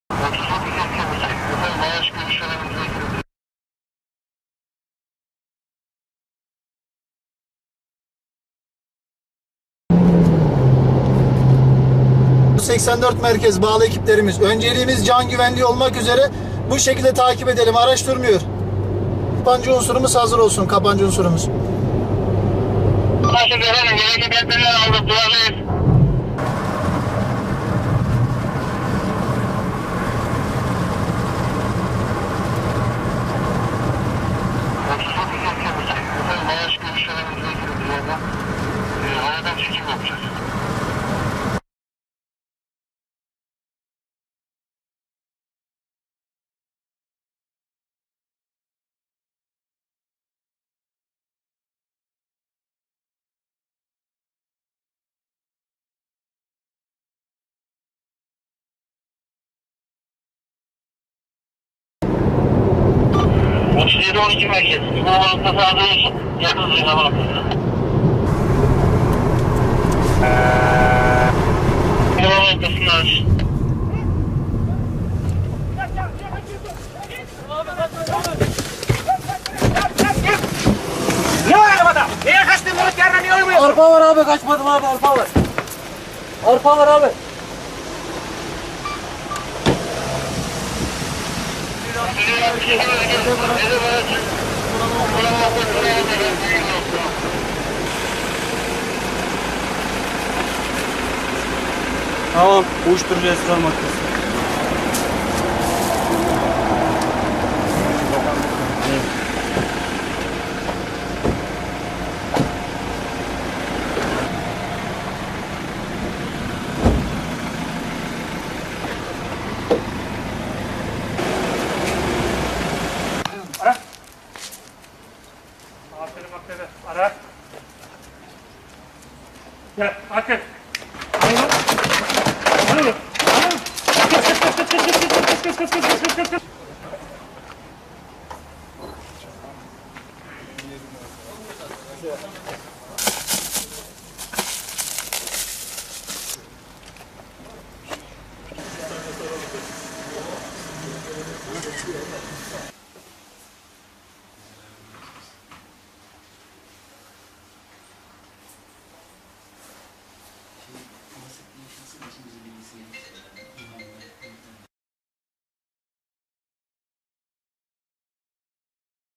Mardin’de Helikopter Destekli Uyuşturucu Operasyonu
Mardin İl Emniyet Müdürlüğü Narkotik Suçlarla Mücadele Şube ve İstihbarat Şube Müdürlüğü ekipleri tarafından Kızıltepe ilçesi ile Derik Üçyol arası İpekyolu üzerinde helikopter destekli yapılan uyuşturucu operasyonunda 320 kilo 450 gram olan uyuşturucu madde ele geçirildi.